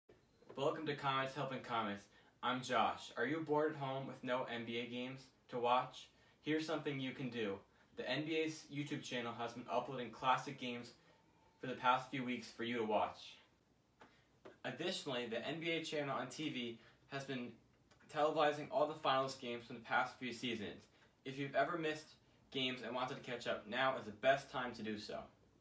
Comets Helping Comets - Catch Up on Classic Basketball
The Comets Helping Comets Projects features HHS Journalism students offering advice and ideas to help our community get through this challenge.